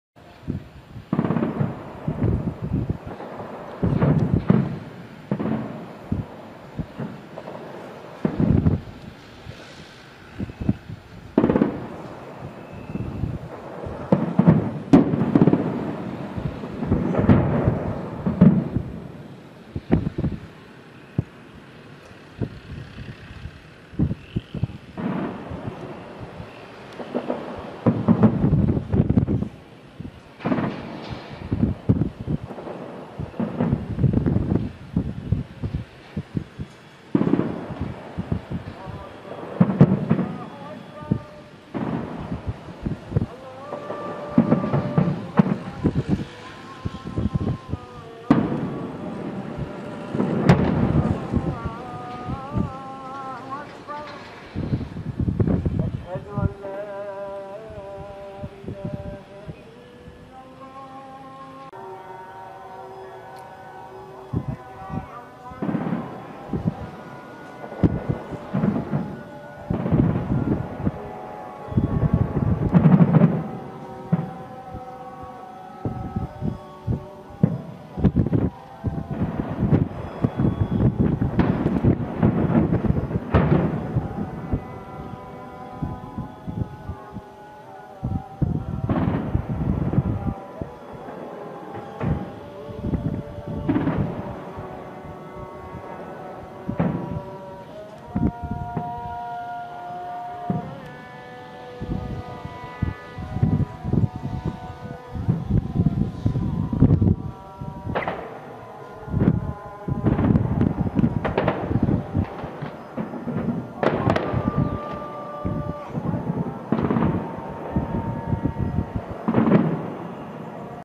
شام حمص الحولة القصف العنيف التي تتعرض له المدينة 25 8 2013 جـ1
مراسل شبكة شام ميسرة الحيلاوي رصد تصاعد الدخان جراء القصف العنيف على مدينة الحولة في ريف حمص.